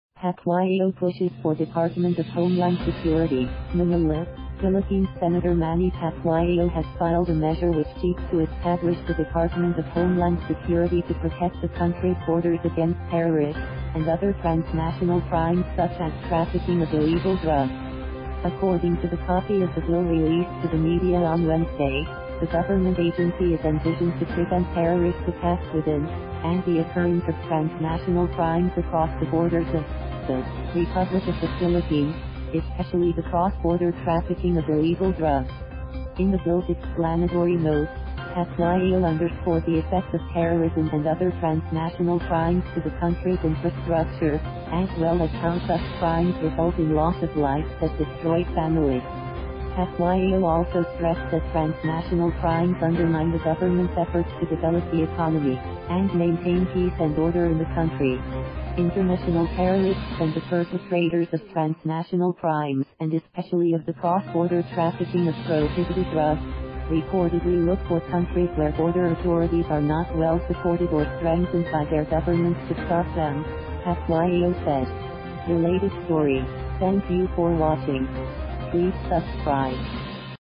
Pacquiao pushes for Department of Homeland Security
MANILA, Philippines — Senator Manny Pacquiao has filed a measure which seeks to establish the Department of Homeland Security to protect the country’s borders against terrorists and other transnational crimes such as trafficking of illegal drugs. According to the copy of the bill released to the media on Wednesday, the government agency is envisioned to “prevent terrorist attacks within, and the occurrence of transnational crimes across the borders of, the Republic of the Philippines, especially the cross-border trafficking of illegal drugs.” In the bill’s explanatory note, Pacquiao underscored the effects of terrorism and other transnational crimes to the country’s infrastructure, as well as how such crimes result in loss of lives that destroy families. Pacquiao also stressed that transnational crimes undermine the government’s efforts to develop the economy and maintain peace and order in the country. “International terrorists and the perpetrators of transnational crimes and especially of the cross-border trafficking of prohibited drugs, reportedly look for countries where border authorities are not well-supported or strengthened by their governments to stop them,” Pacquiao said. ) RELATED STORY:...